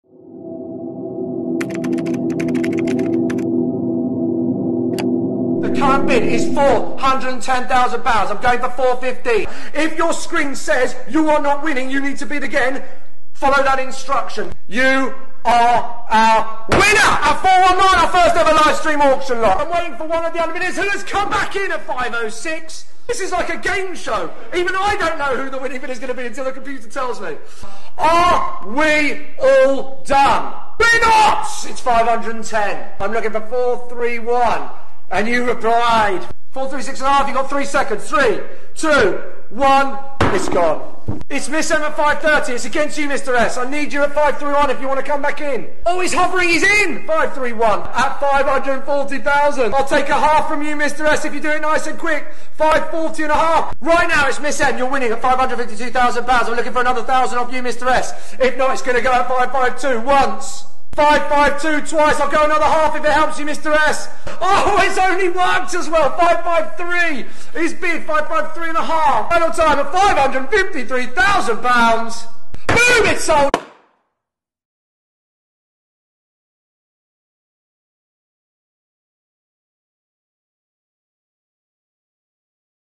Auction House London’s “1 Minute” Highlights from 25th March 2020
Our first ever live stream auction - these are the ultra short highlights just to give you a very quick idea of what having a live auctioneer like Andrew Binstock will do versus a standard online auction.